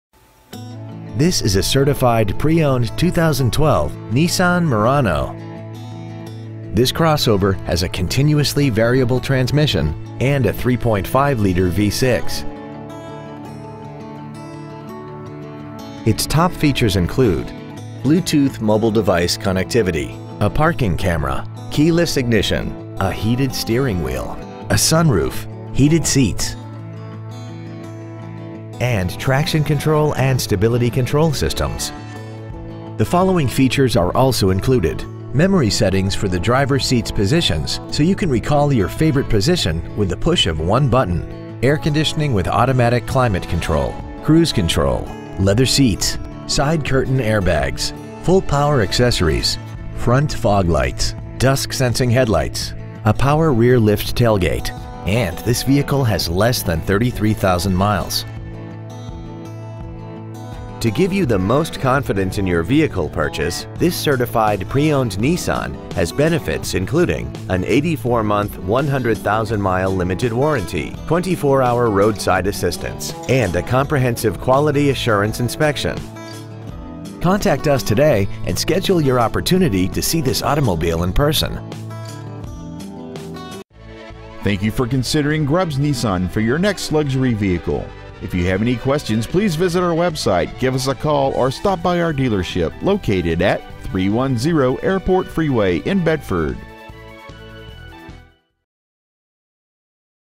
Preowned 2012 Nissan Murano Dallas TX
We have been honored to serve the Bedford TX area , we promise that your experience at our dealership will exceed your expectations ! Year : 2012 Make : Nissan Model : Murano Engine : 3.5L V6 Trans . : CVT (Continuously Variable) Exterior : Platinum Graphite Metallic Miles : 32,495 Interior : Black Stock : N11620A Grubbs Nissan Airport Freeway Bedford , TX 76022